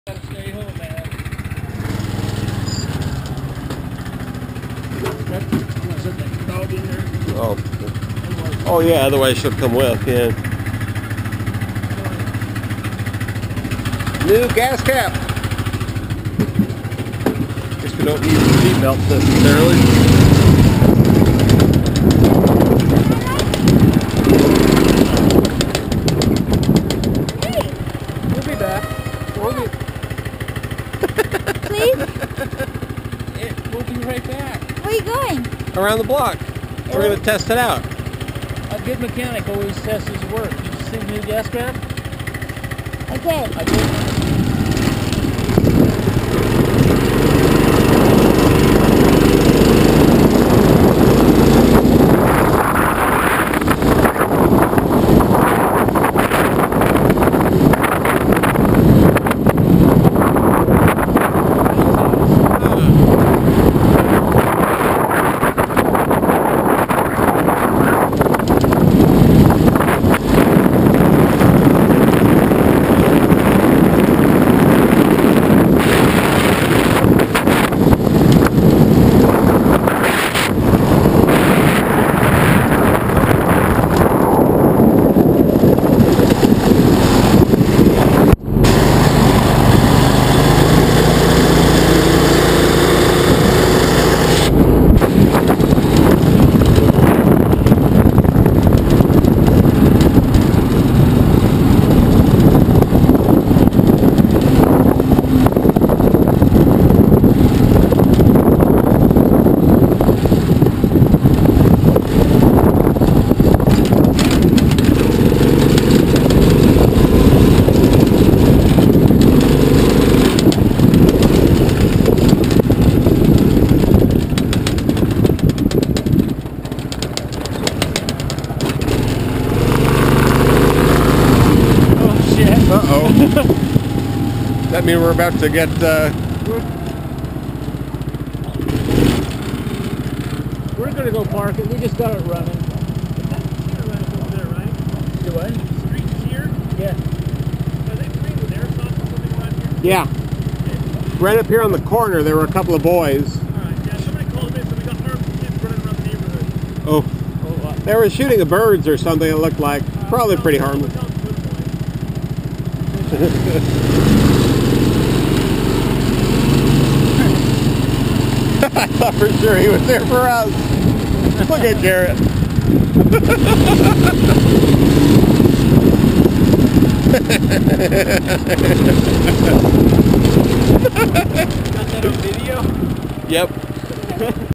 Road testing the home made gas cap
I lost the gas cap to the go cart, so Dave made one out of a block of wood.  At 00:46 you can see how it works out.  Then, at 02:12 the whole trip takes an unexpected turn.